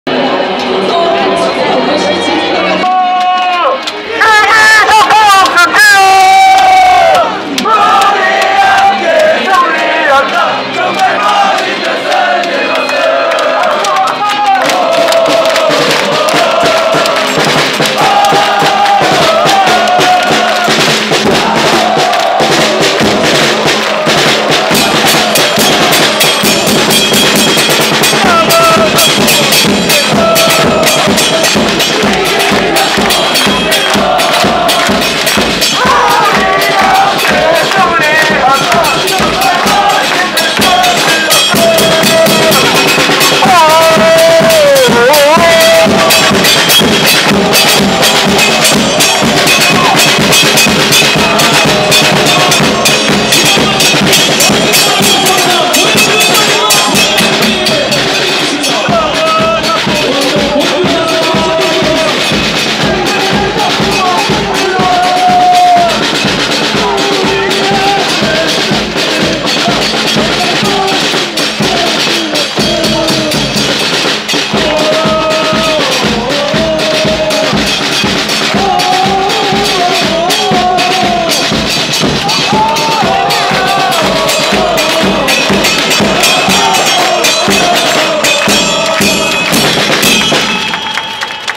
현장응원)20220508 성남fc전 우리함께 스틸러스
2022년 5월 8일 성남FC전 홈 경기

현장악기팀 응원